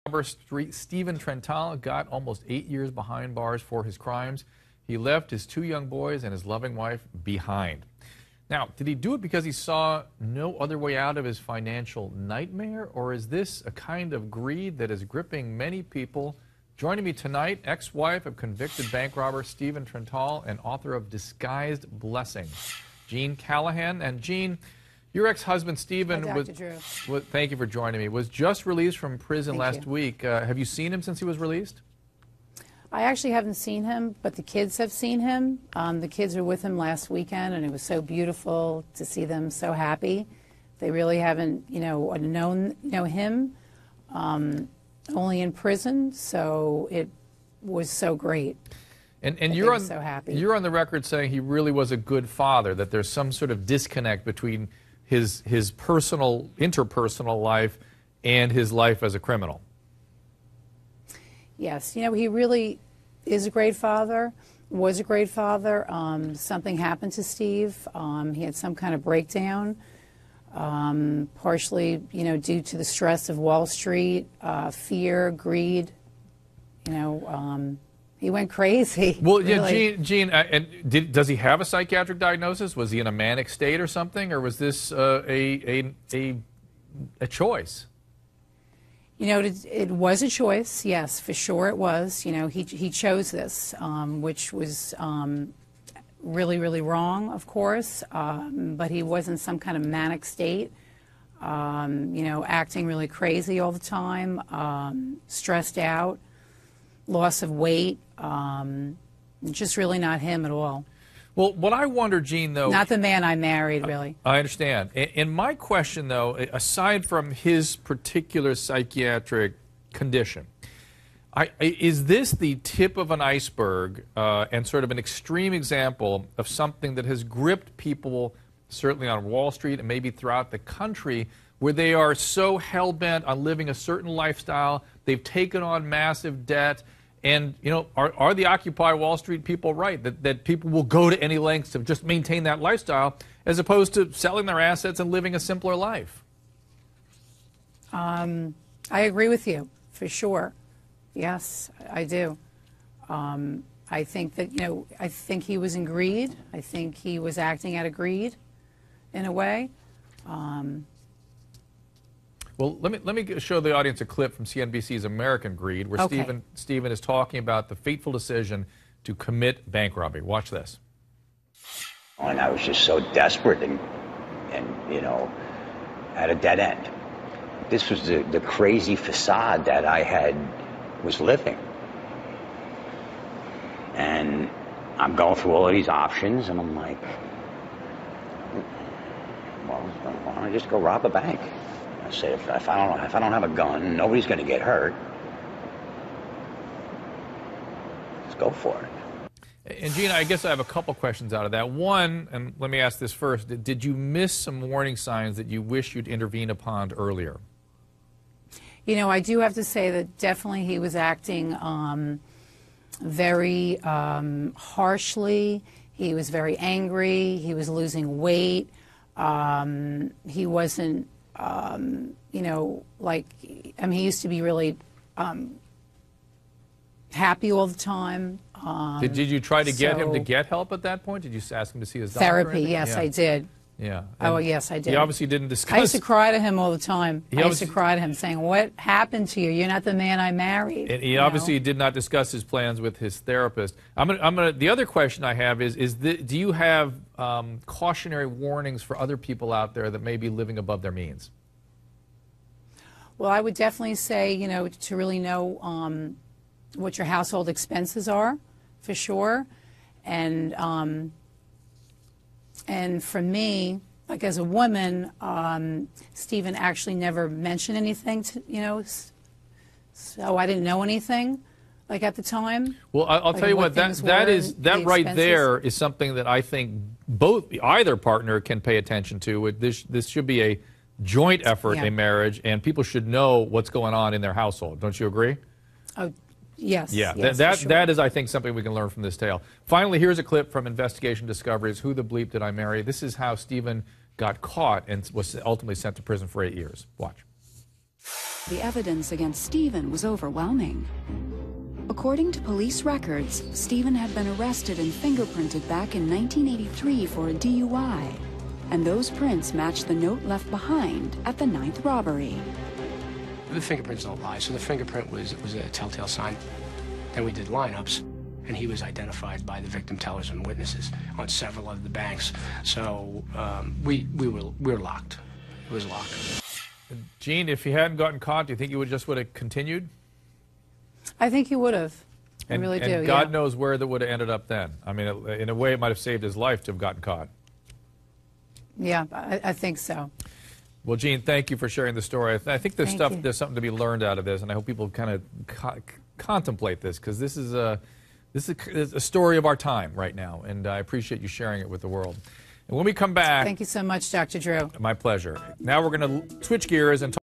Bank robber's ex-wife tells all
A Wall Street broker on hard times turns to crime and ruins life for himself, his wife, and their two kids.